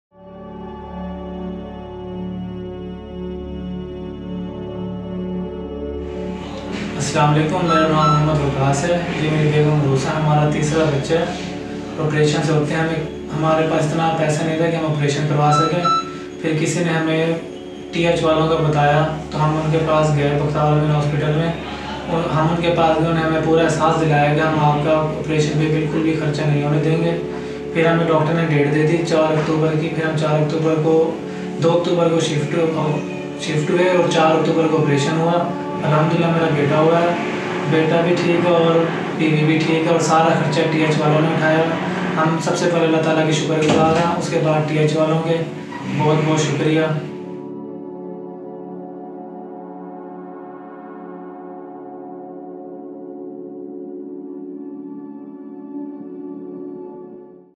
Arousa Bibi's Success Story | Cesarean Section Surgery
Aroosa Bibi, a 23-year-old resident of Multan, was pregnant. Since she previously had three C-sections, the doctor advised her to have an elective Cesarean Section Surgery to avoid complications. Unfortunately, due to poor financial conditions they couldn’t afford the procedure. A friend told them about Transparent Hands and they got their case registered. Aroosa Bibi was admitted to BAMT Hospital where her C-section was performed by Dr. Asim Iqbal on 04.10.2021. Aroosa Bibi was blessed with a baby boy. Both Aroosa and her son were discharged in good health after two days of hospital stay.